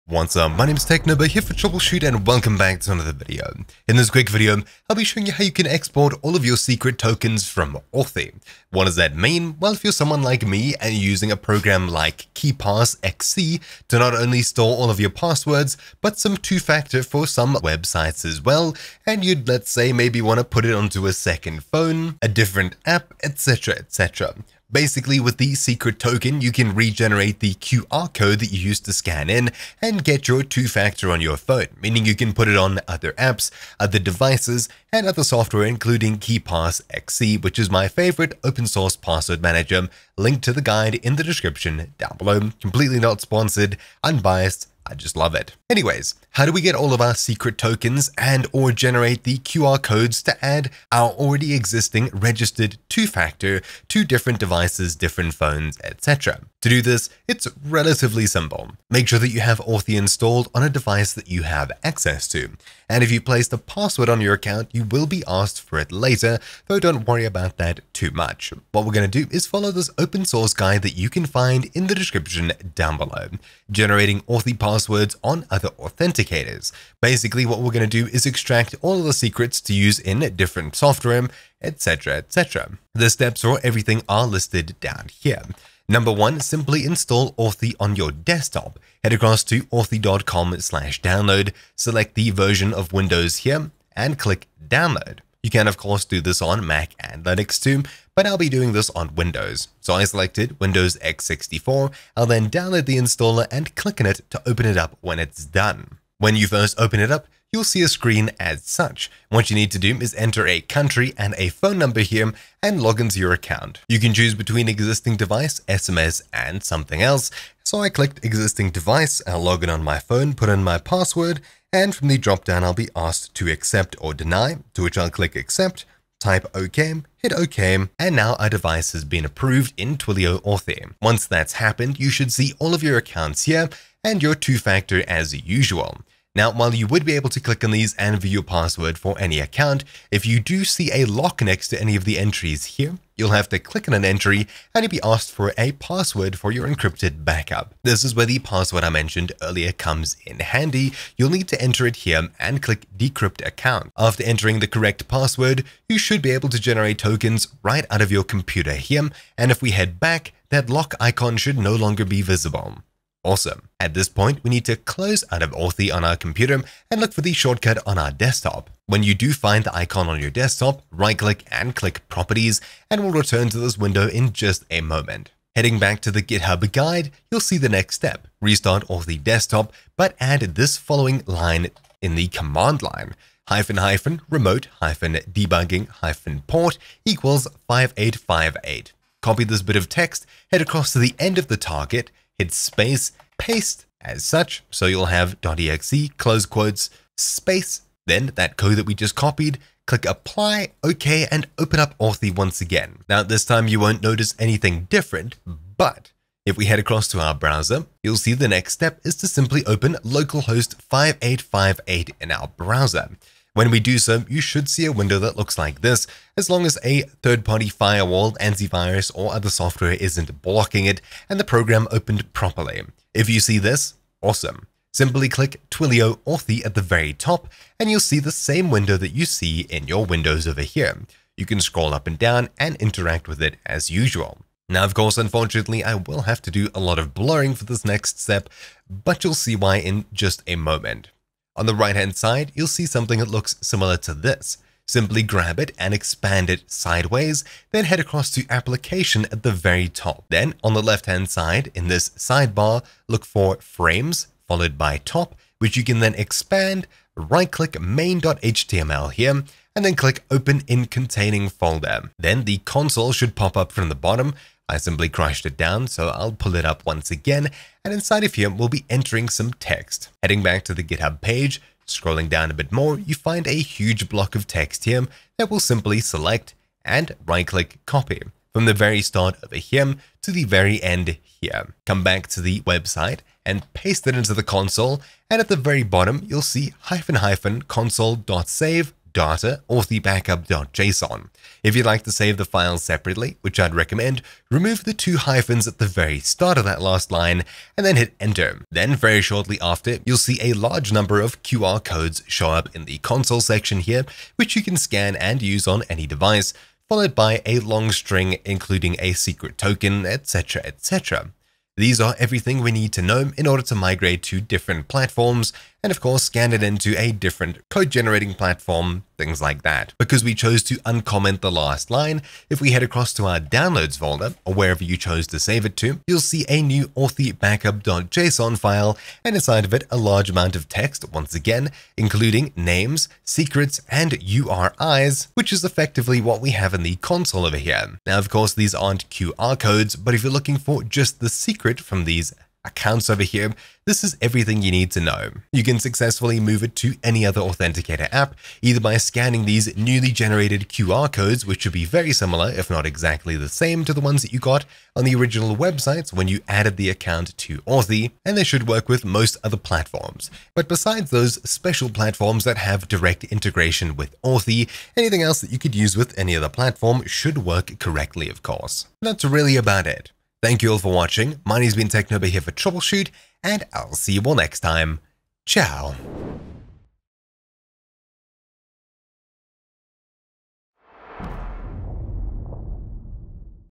Export Accounts/QR Codes/Secrets from Authy | Migration Guide
Want to move 2-factor from Authy to another app or program? Don't want to remove and re-add EVERYTHING? Well, you can simply generate QR codes to scan and use in other software, as well as get the secret to generate 2-factor codes in other software super simply! This guide shows you everything you need to know. For me, I was moving from Authy to KeePass as I use it for everything else.

Timestamps:
0:00 - Explanation
0:46 - How to recover QR codes/Secrets from Authy
1:25 - Install Authy on Desktop
2:10 - Enter password for backup (if set)
2:43 - Modify Authy desktop shortcut (or launch with argument)
3:22 - Open Authy in Browser (Chrome Preferred)
3:54 - How to get secrets/QR Codes from Authy
5:20 - Simple way to get secrets (After removing 2 lines from code)